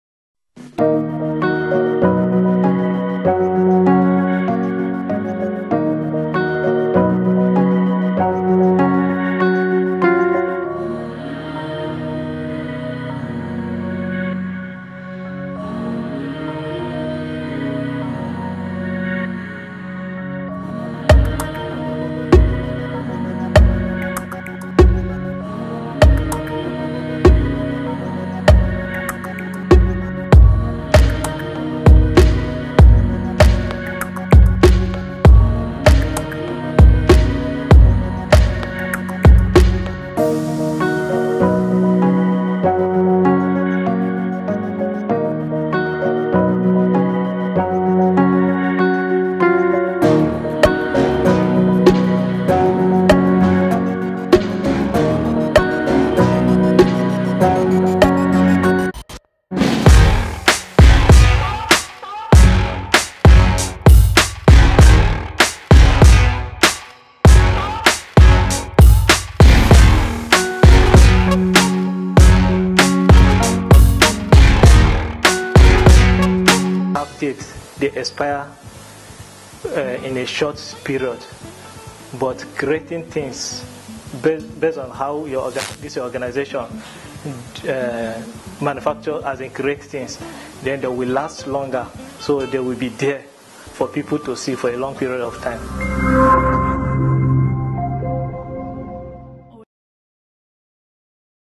BULUS BITRUS
BULUS BITRUS a graphic Artist and an NCE holder share his happiness with Dazzle at the just concluded Dazzle Carpentry Training.